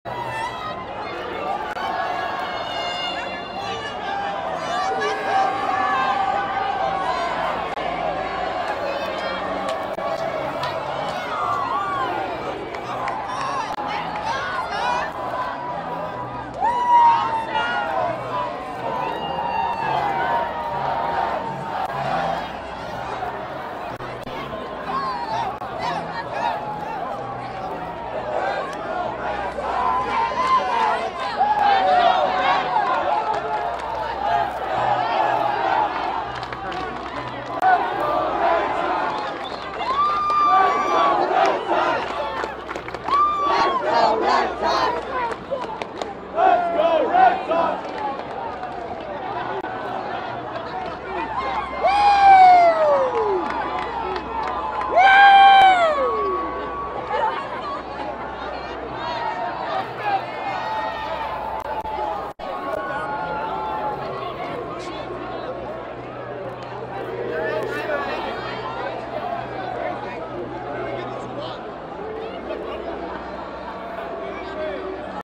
Boston Red Sox fans take to the streets to celebrate World Series win
Fans of the Boston Red Sox took to the streets outside Fenway Park after the team won the World Series against the Los Angeles Dodgers in Game 5.